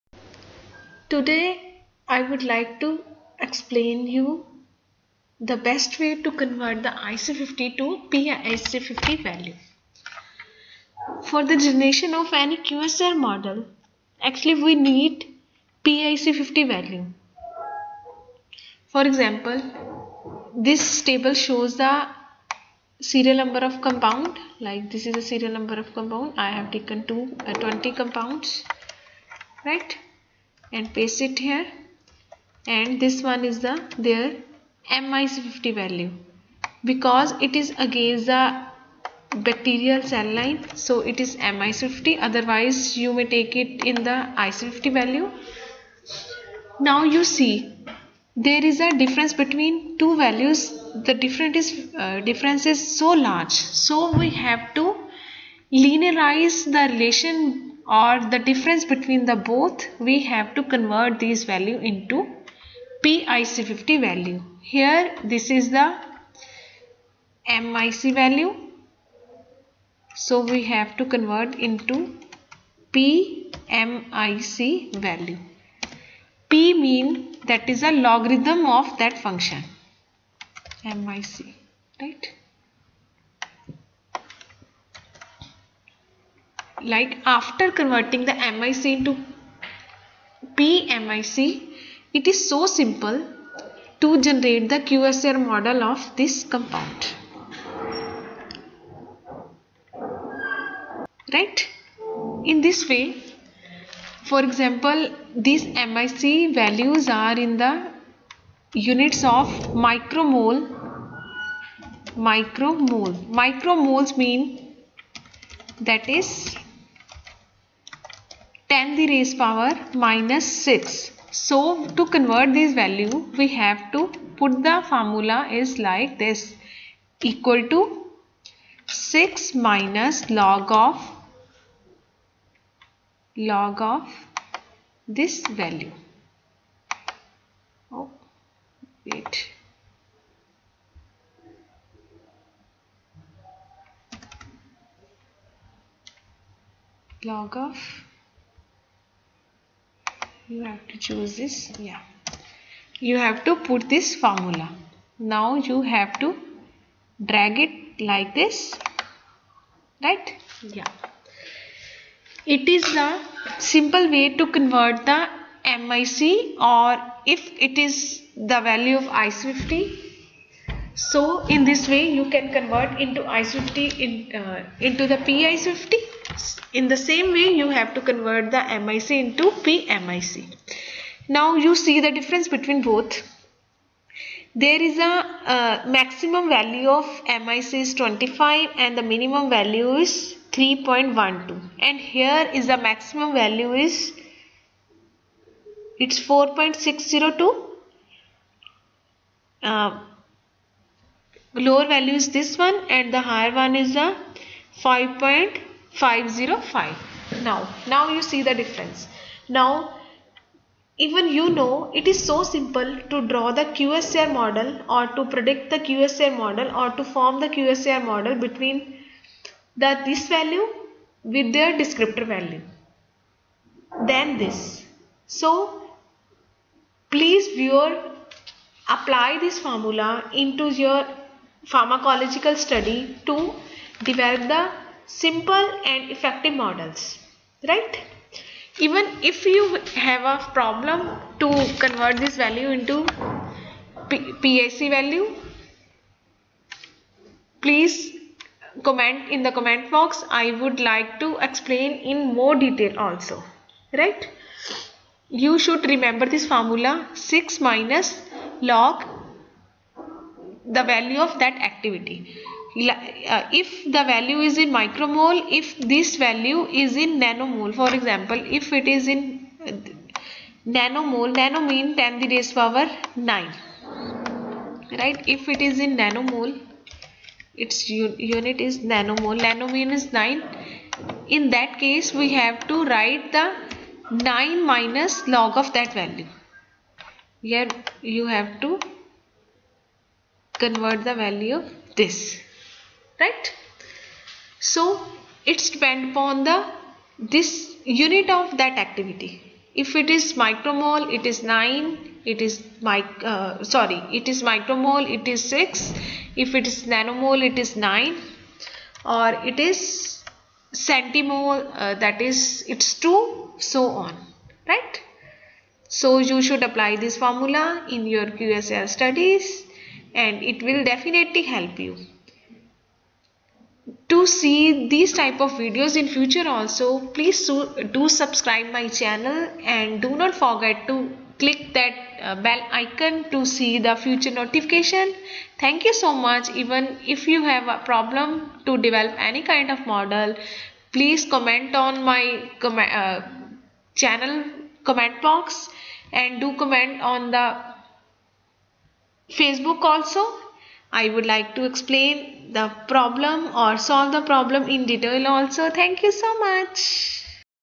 Convertion of IC50 TO logarthmic functions
Best way to convert IC50 or MIC into pIC50 or pMIC.